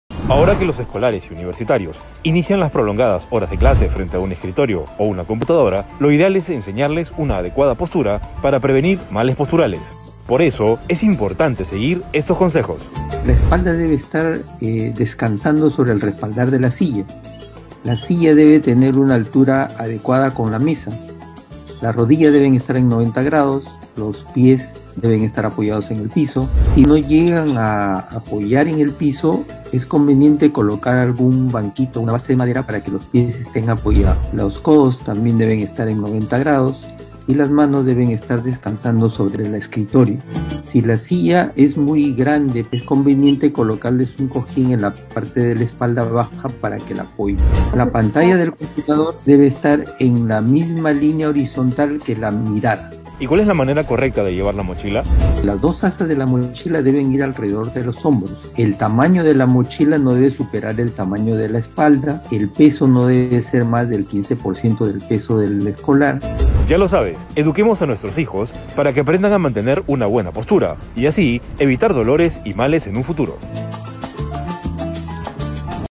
Aprendamos posturas correctas para niños y adolescentes en etapa escolar
SaludEnAcción - Edición 4 de abril
Material elaborado por el Ministerio de Salud